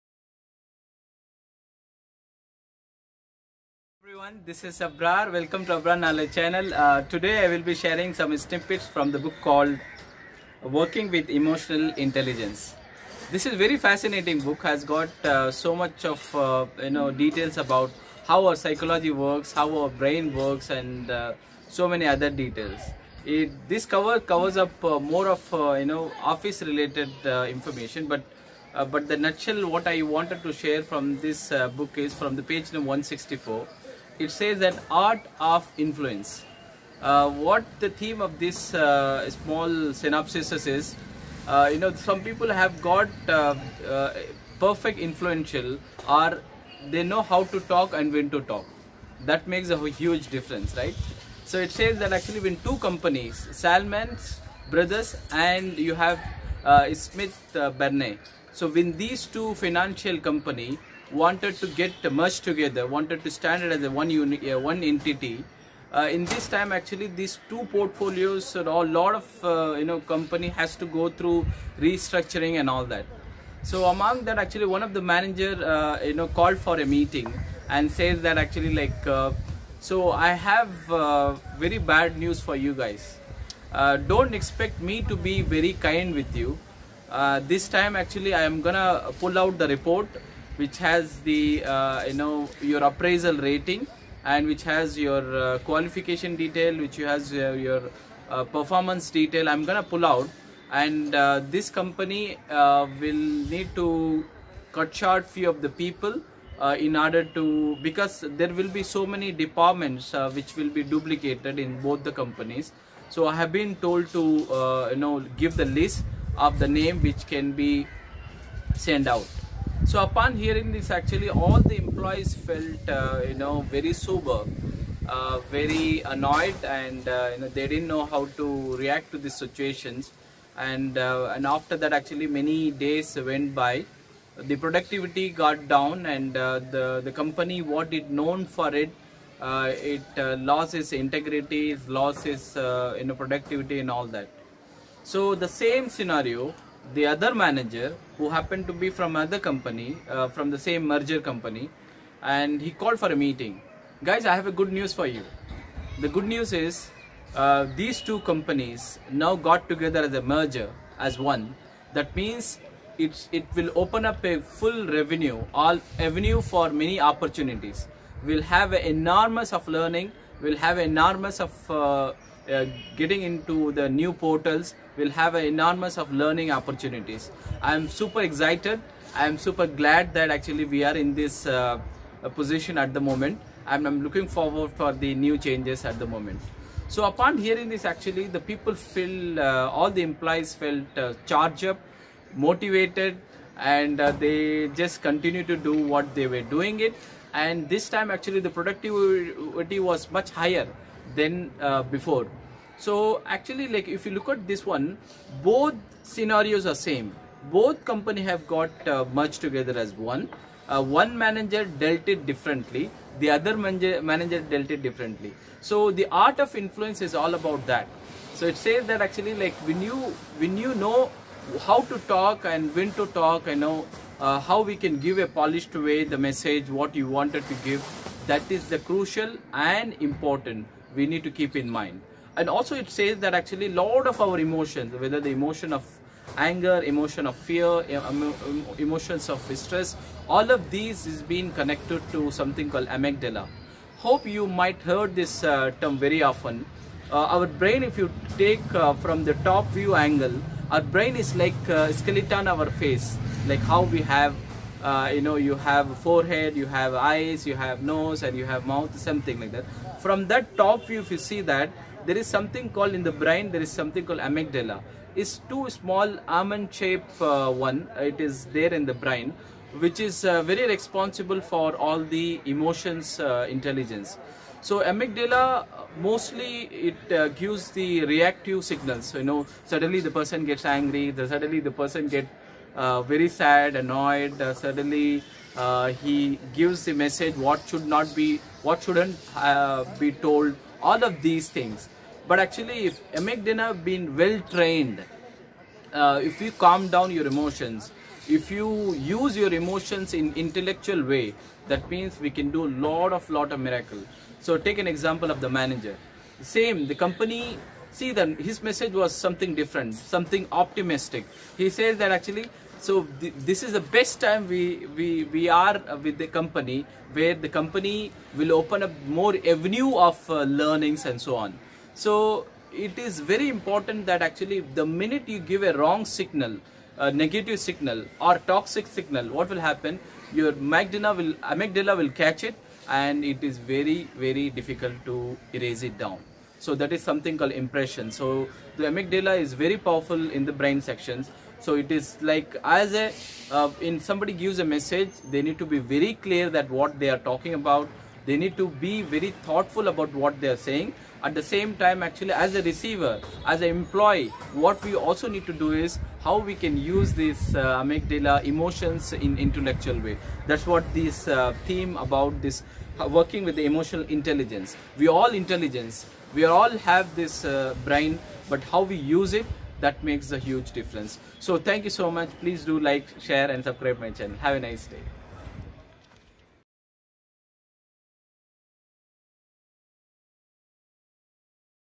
How to use amygdala effective to manage emotions effectively
Working With Emotional Intelligence is management book and core theme of this book is control your Amygdala, when you control Amygdala, lot of emotions such as Anger, Stress, fear can be overcome easily.